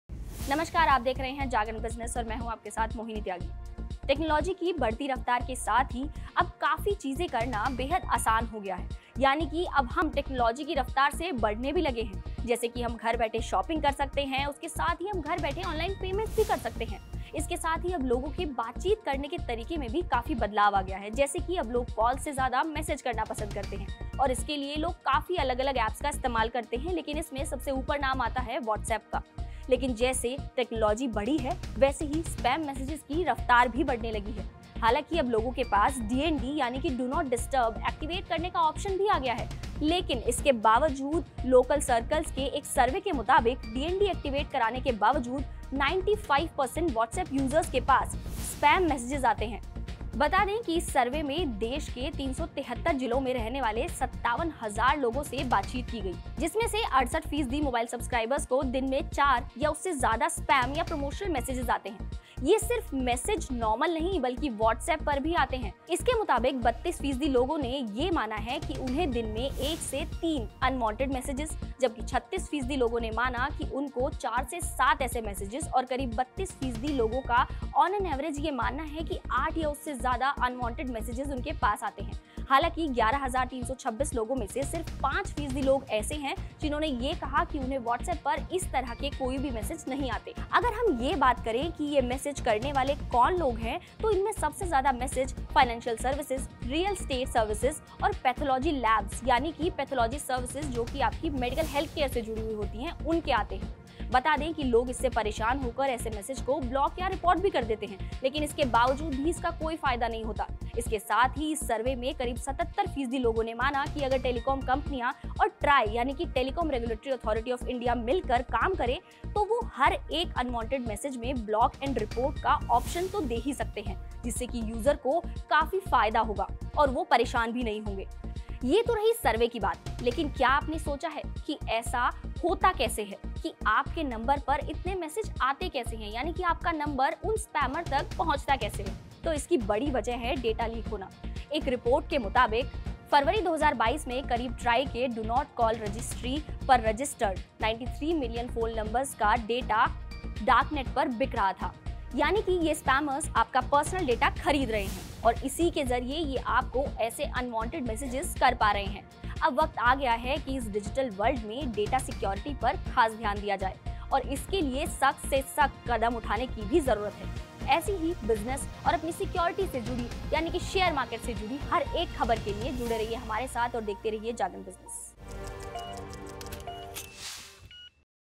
कैसे रुकेगा Spam का Scam?
देश में Spam Messages के द्वारा किये जा रहे Scams लगातार बढ़ते जा रहे हैं, लाखों कोशिशों के बाद भी आपके फ़ोन पर spam messages आना बंद नहीं होते तो इस वीडियो को अंत तक जरूर देखे और अपने आप को Spam messages और उनसे होने वाले Scams से खुद को बचाये !